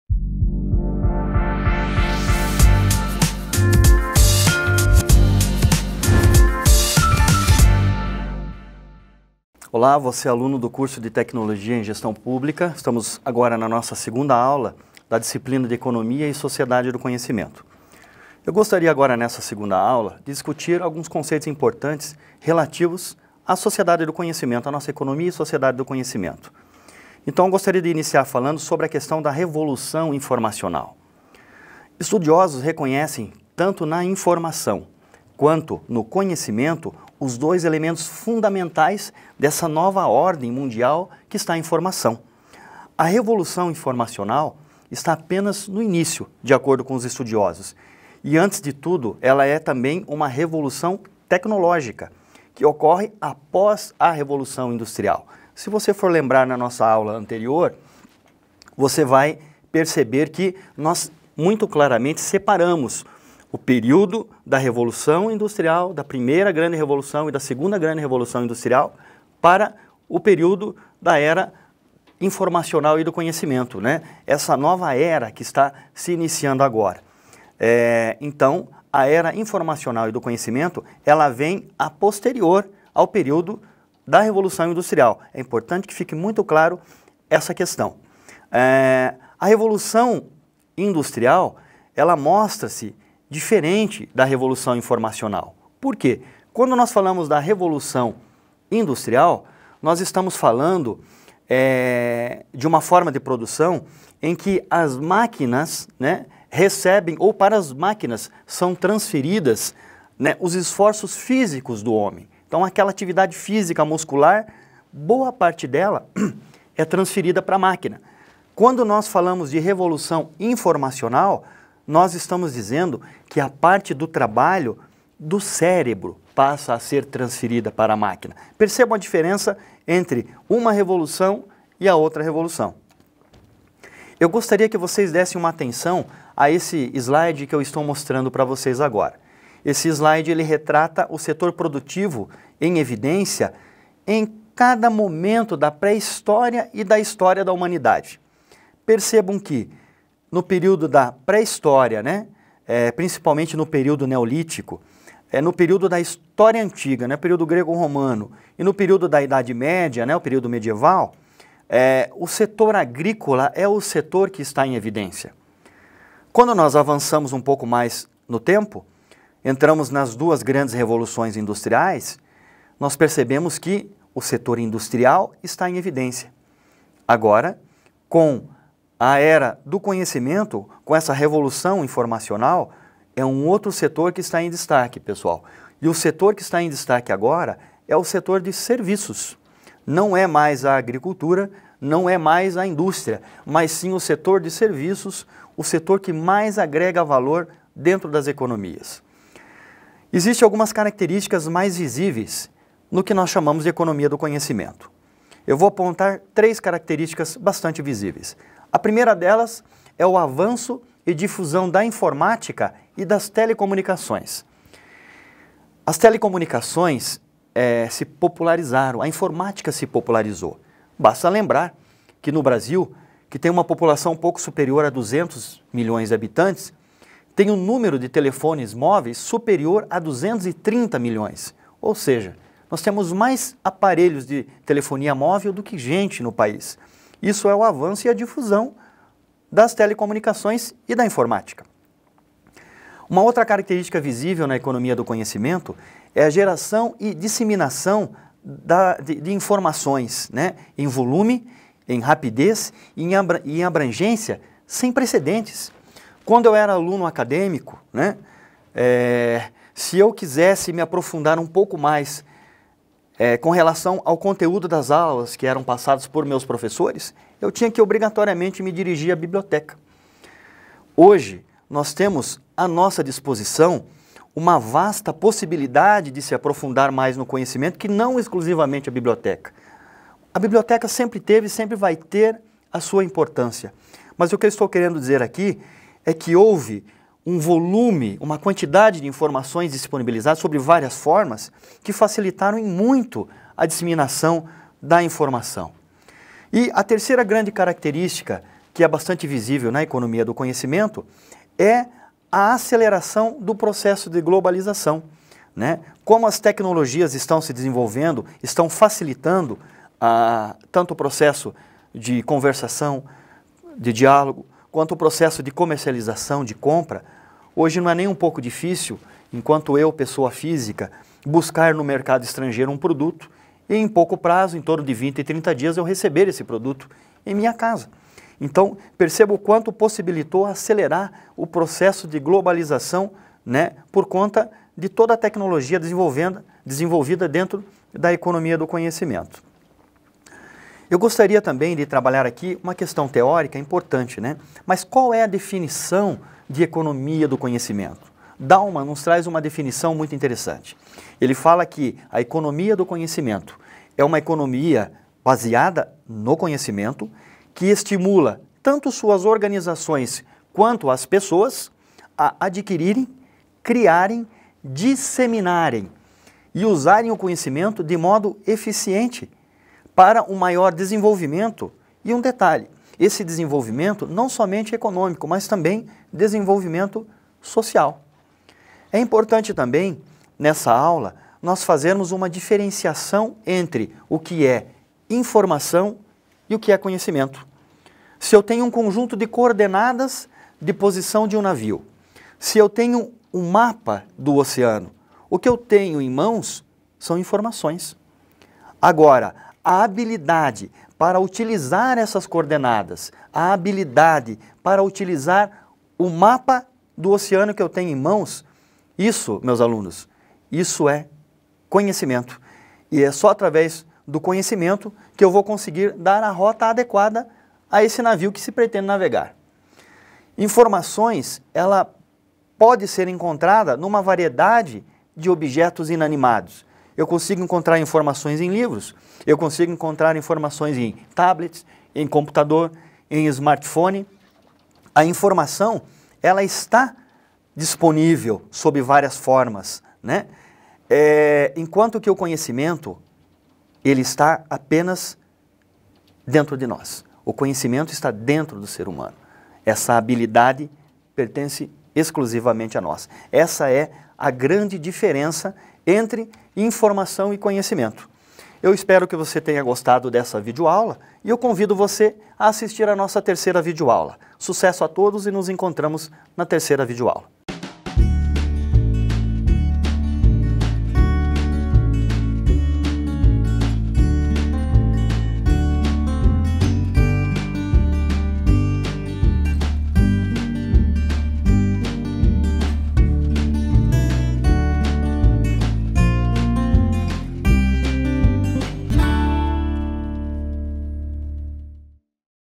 [UVPR] Características da Economia e Sociedade do Conhecimento
[UVPR] Características da Economia e Sociedade do Conhecimento com o professor Amarildo Hersen para a disciplina de Economia e Sociedade do Conhecimento para o Curso de Graduação em Tecnólogo em Gestão Pública. 2